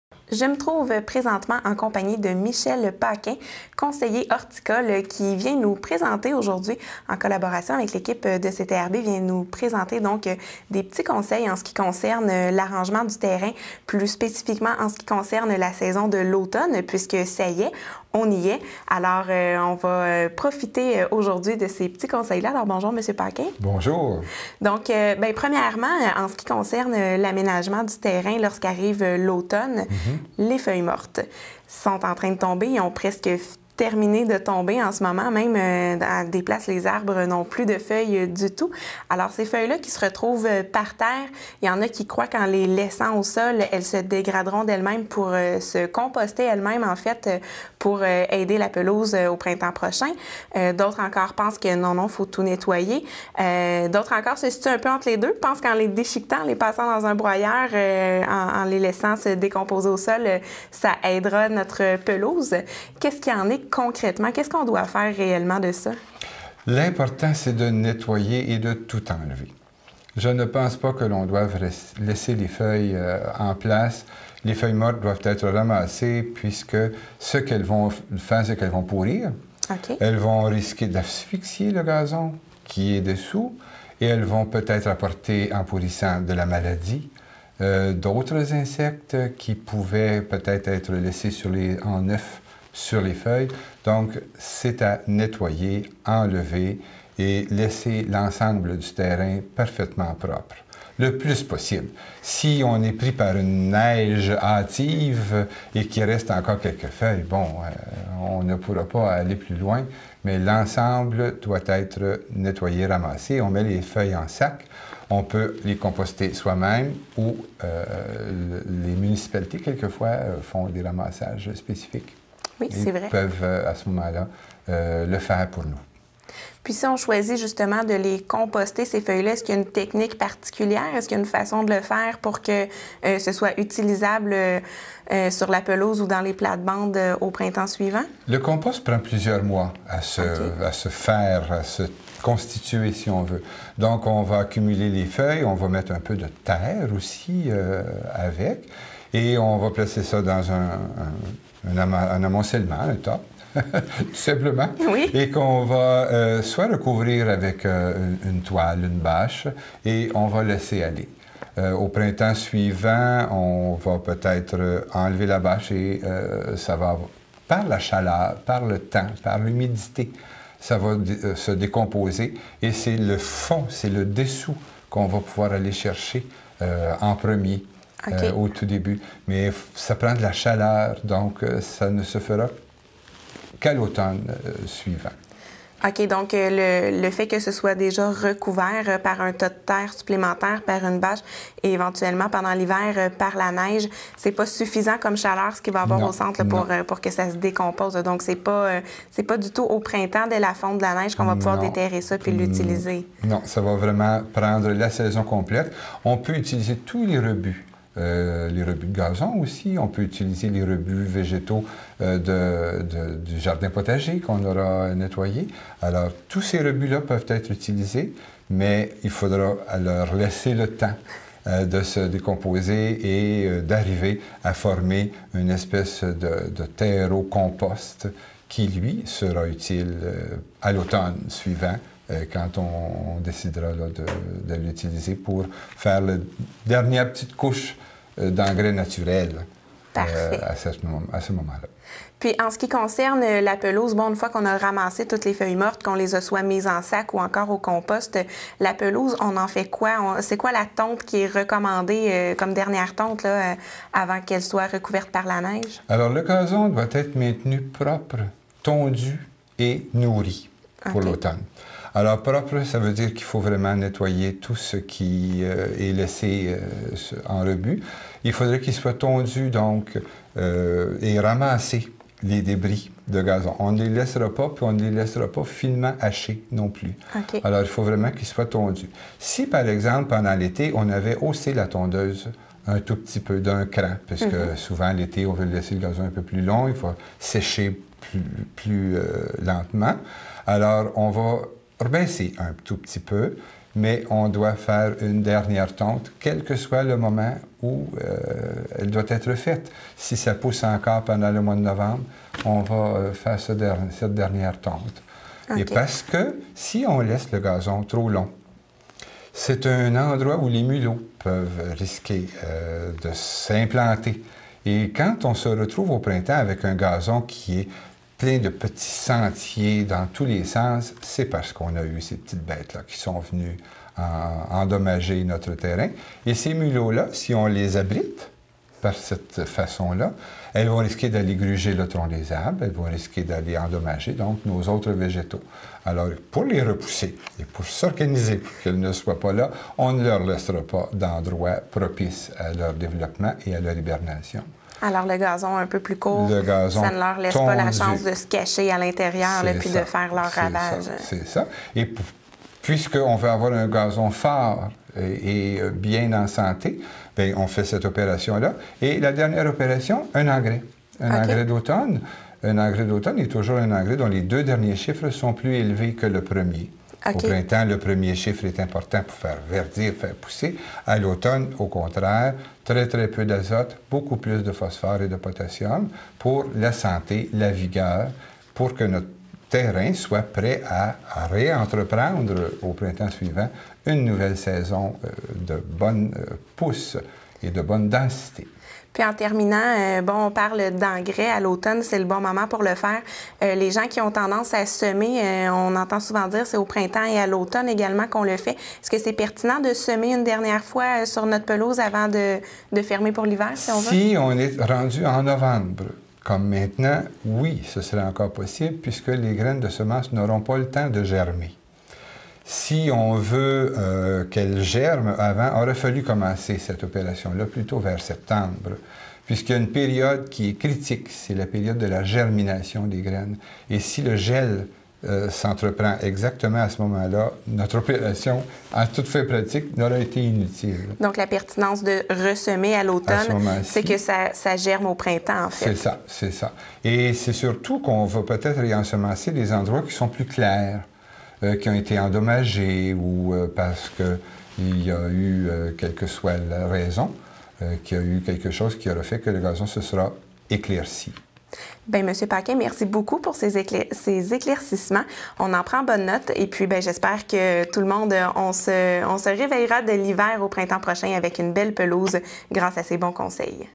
Les chroniques horticoles de Michel Paquin no. 1
Mr Paquin nous parle de l'entretien de nos terrains face à l'arrivée de l'hiver